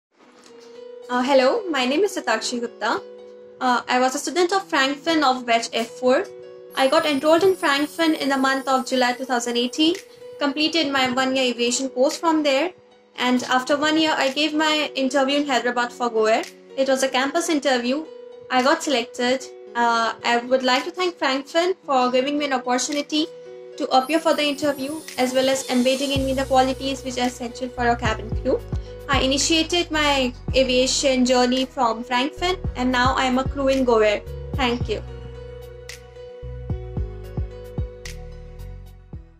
Frankfinn Alumni Testimonial | Shatakshi Sandeep Gupta | Placement - Cabin Crew in GoAir
In this uplifting student testimonial video Ms. Shatakshi Sandeep Gupta, a former student at Frankfinn Institute of AirHostess Training Institute Nagpur Centre, Maharashtra, shares her overwhelming positive experience at Frankfinn. She shares her experience of campus interviews after she completed her one year aviation course at Frankfinn.

Ms. Gupta has been successfully  placed in GoAir as cabin crew after completing her training at Frankfinn Institute of AirHostess Training.